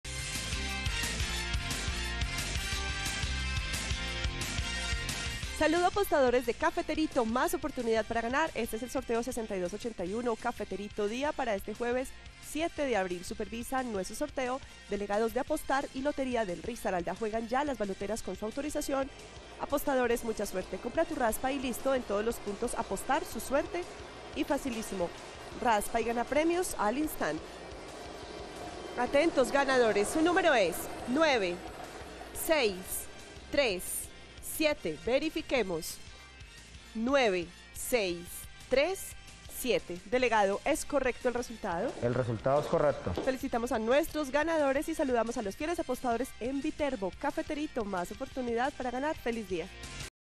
EL CAFETERITO TARDE Resultado del JUEVES 07 de abril de 2022 NÚMERO GANADOR ✅🥇🔥💰
Hola buenas tardes, te presentamos los resultados de las loterías y chances en Colombia, vamos con el Resultados del Chance Cafeterito Tarde del JUEVES 07 de abril de 2022.

el Resultado Lotería de Bogotá JUEVES 07 de abril de 2022
Resultado Chontico noche JUEVES 07 de abril de 2022

Resultados Lotería de Bogotá, Lotería del Quindío y sorteo Astro Luna